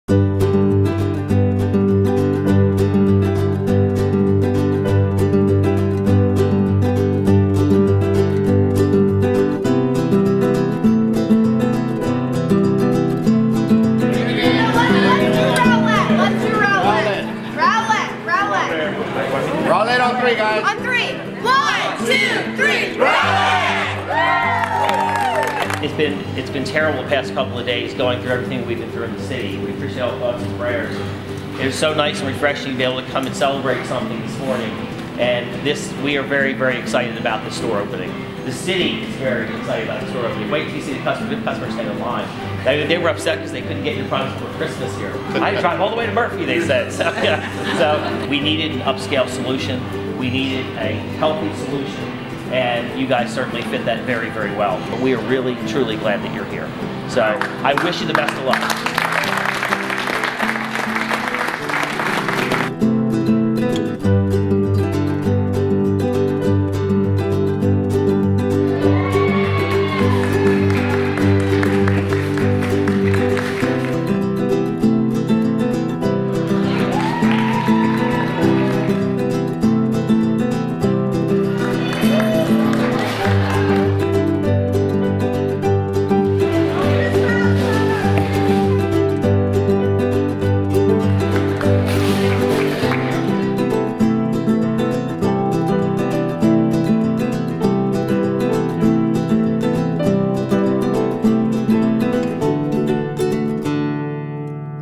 Sprouts Farmers Market Grand Opening - Rowlett, Texas (Dec. 30, 2015)
Sprouts Farmers Market is now open in Rowlett, Texas. Located at 3001 Lakeview Parkway.